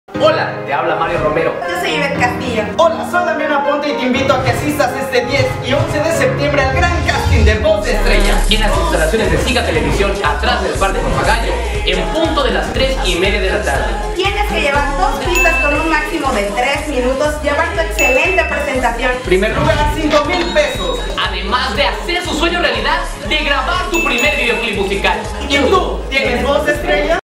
Promo Voz de Estrella
Invitación al Casting a realizarse el 10 y 11 de septiembre en SigaTV en Acapulco, Gro.  a un costado del parque Papagayo.